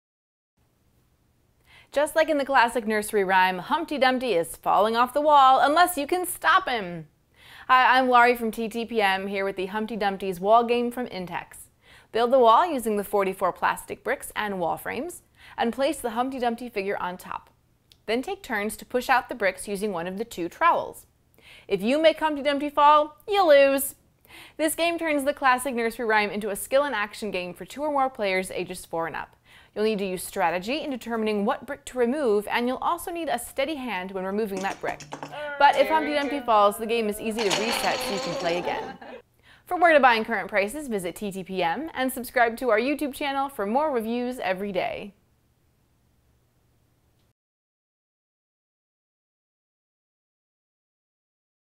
Humpty Dumpty's Wall Game from Intex Entertainment
Humpty Dumpty’s Wall Game from Intex. Skill-and-action game for ages 4 and up. Similar gameplay to Jenga in that players take turns removing bricks from a wall without making Humpty fall. The winner is the player who doesn’t make the Humpty Dumpty figure fall. For full review and shopping info

Product Info: Humpty Dumpty’s Wall Game is a skill-and-action game for young players where you take turns removing bricks from the wall without making Humpty fall.